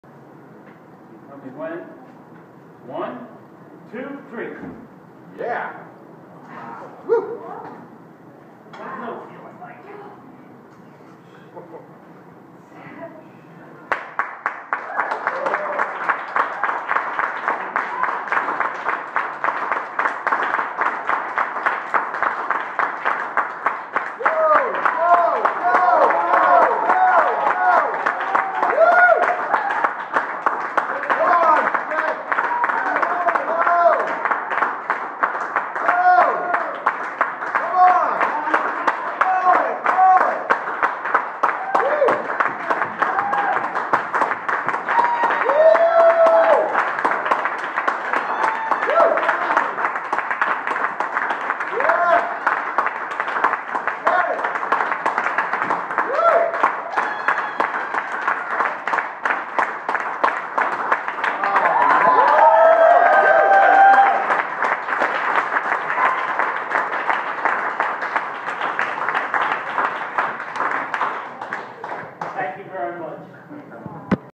Chris "Wonder" Schoeck
My friend and protege Chris "Wonder" Schoeck performing his signature scroll at the 2013 Coney Island Film Festival. This feat was performed during the Q&A session following the screening of "Bending Steel". To learn more about this award winning feature documentary